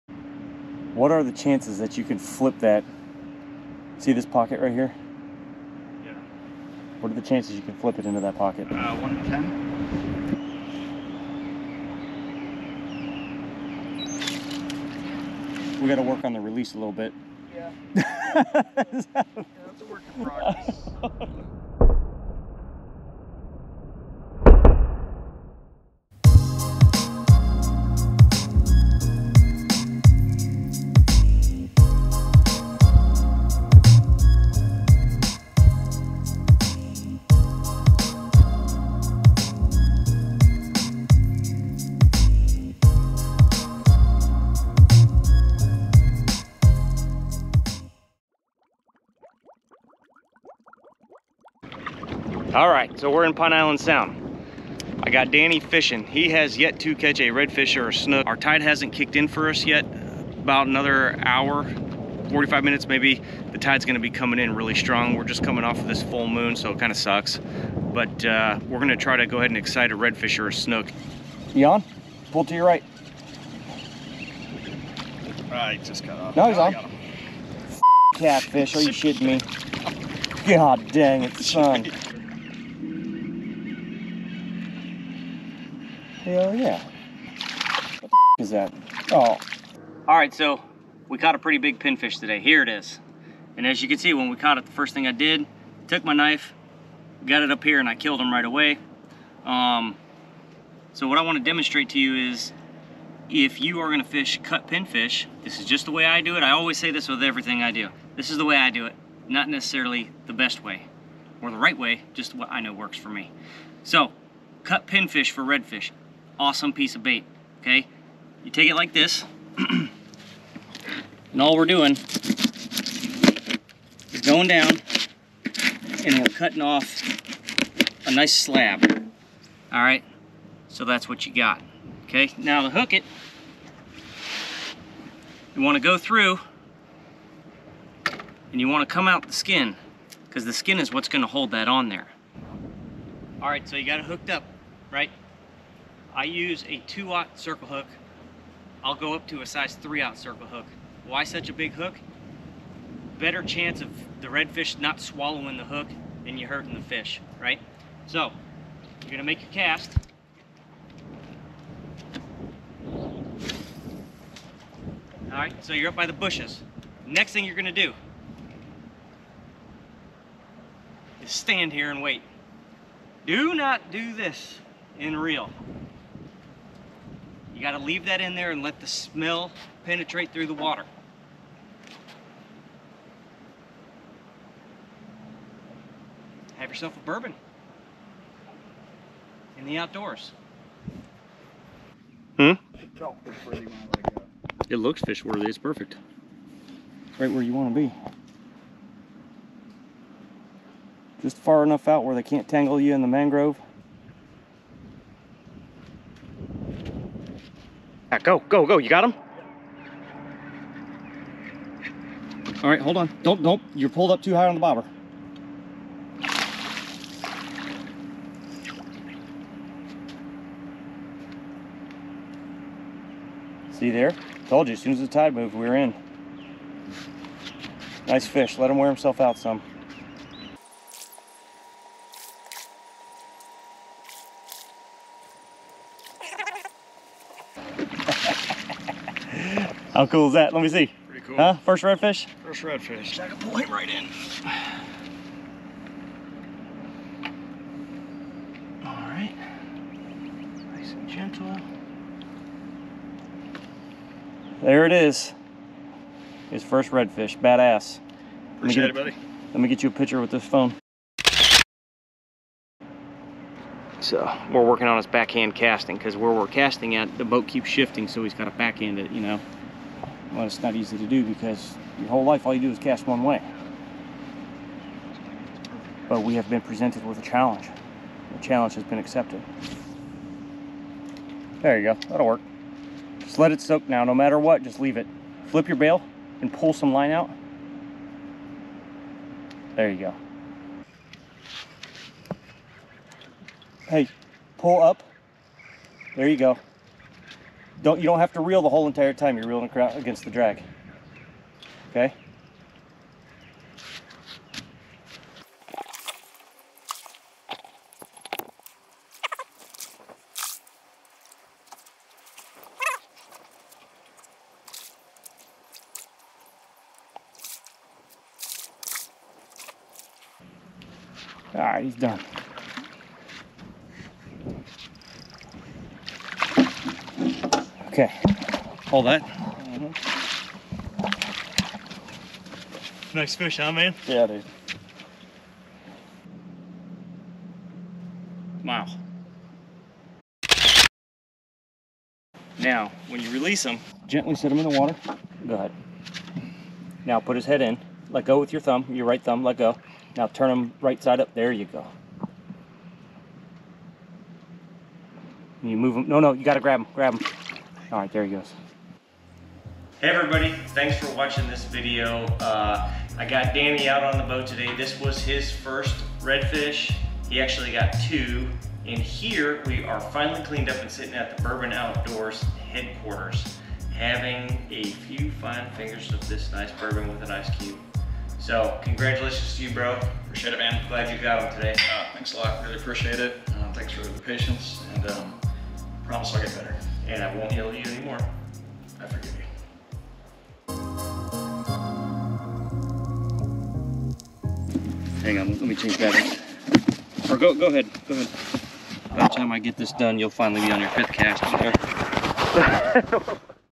Guiding the New Guy to His First Redfish Catch
Day on the water with my good buddy Danny who is new at fishing.  A little coaching was necessary but he landed two awesome reds.  Danny is also coaching me on video editing and production and has also put together a few vids for me...like this one!